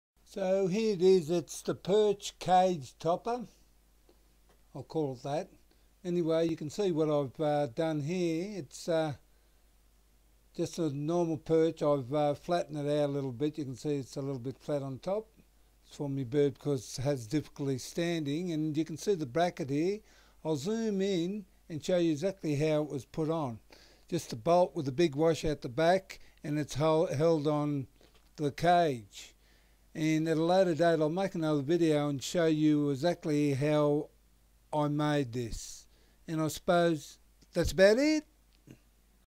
✅OVER THE TOP CAGE PERCH
Made this so our bird is able to perch outside when let out of cage.
Diy bird toys For cockatiels: parrots: Budgies: parakeet: conures: lovebirds: redrump parrot: Grass parrot SEE MORE PERCH IDEAS